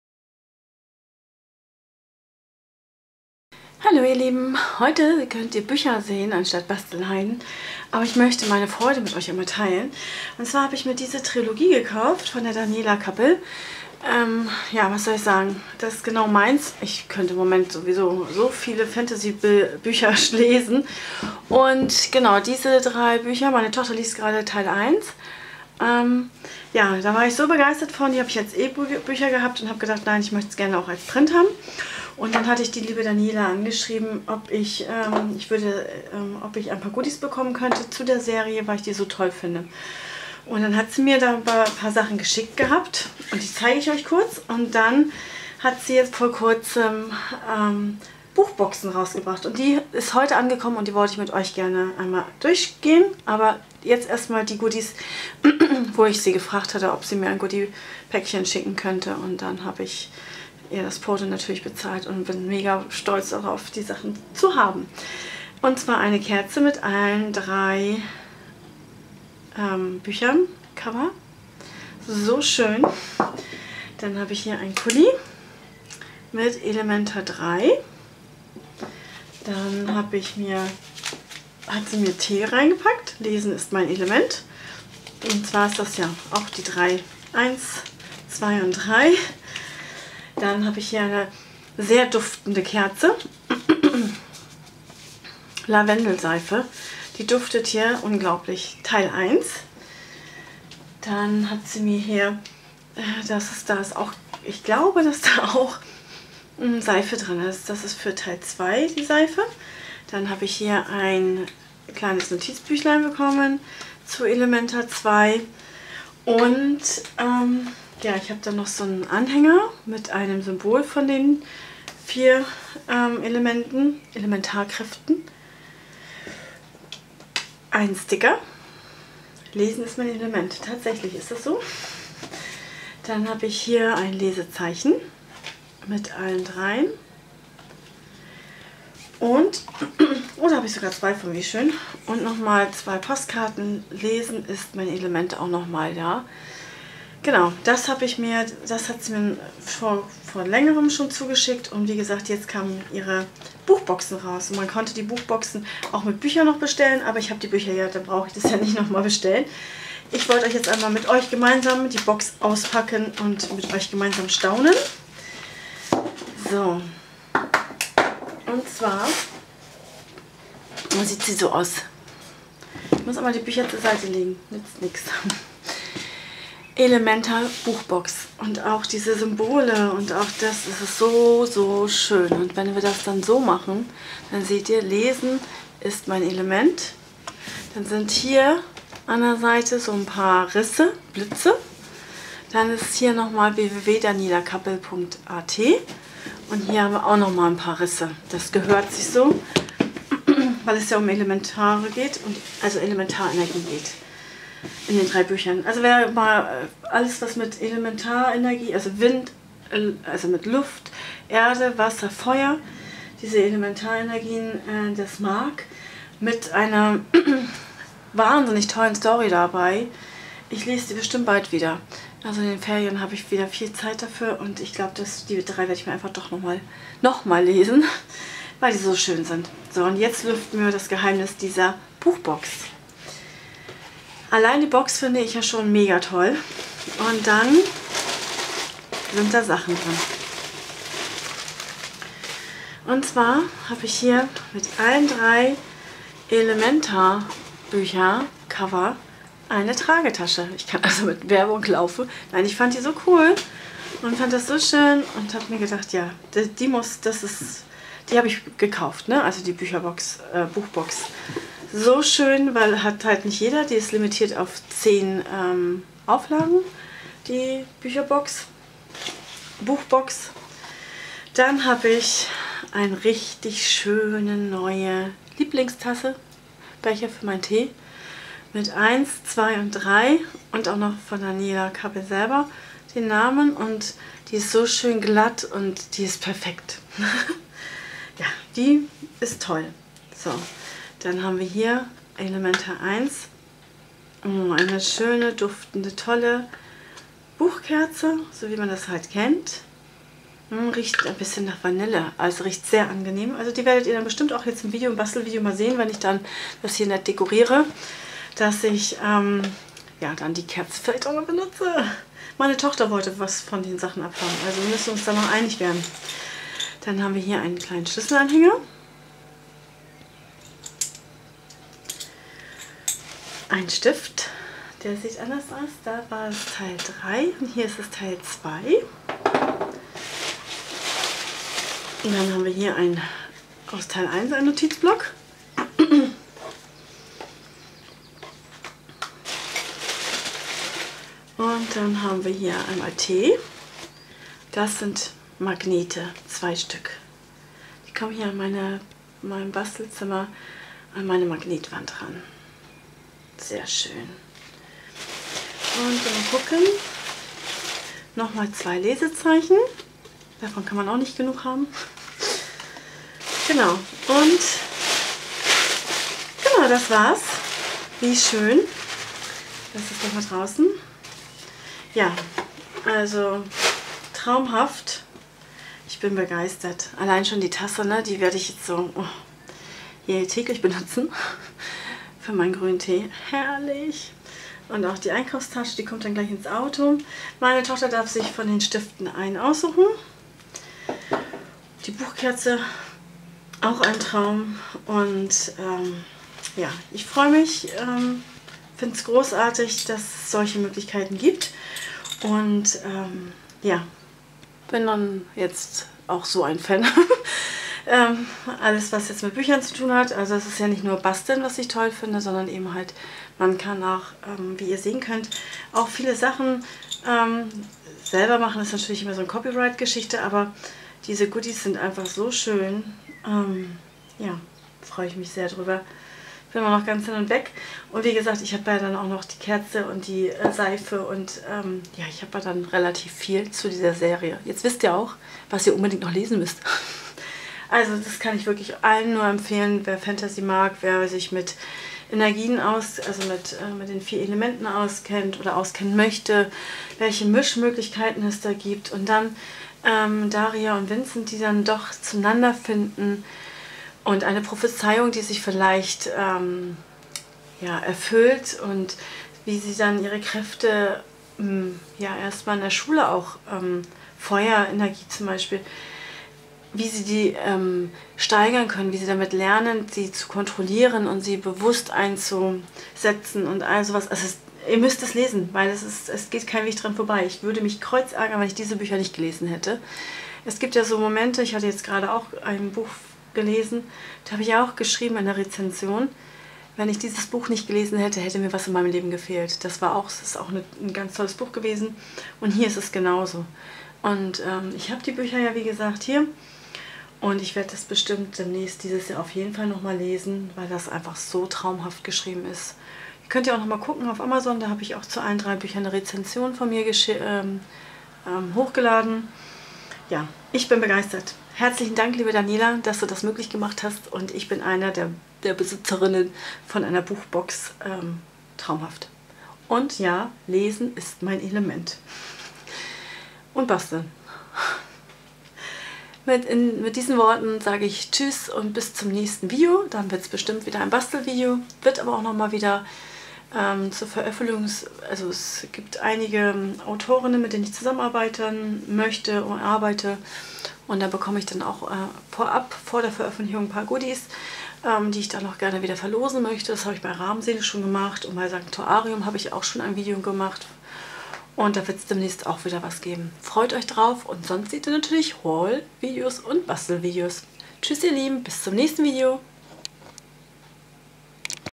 Unboxing +++ Buchbox Elementa I bis III
++Werbung++

heute einfach mal ein Unboxing Video.
Ich habe mir bei Daniela Kappel eine wunderschöne Buchbox gekauft. Und die packen wir gemeinsam aus.

Ach, die Goodies sind soooo schön...

Kreative Grüße
eure Karina